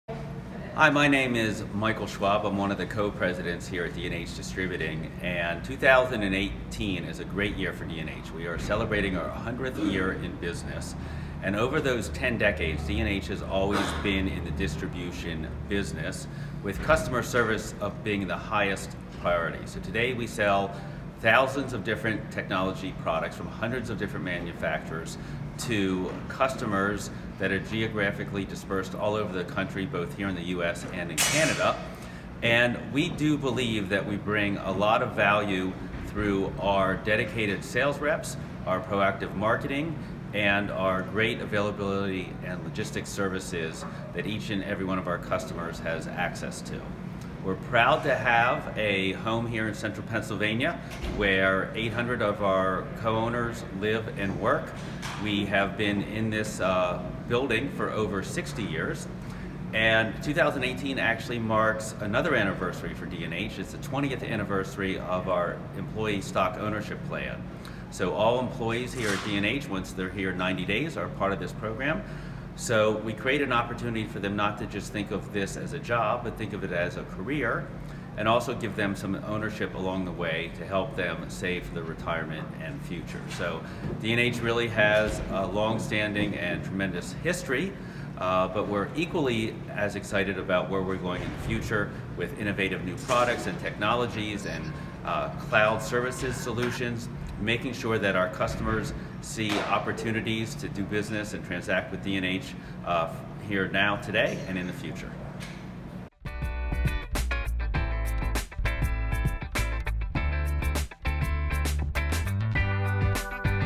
D&H Distributing marks 100th year
Take a look at a number of electronic items that have been popular through the years. All of them have been distributed by D&H Distributing, a Harrisburg, Pa. company that is marking its 100th year in business.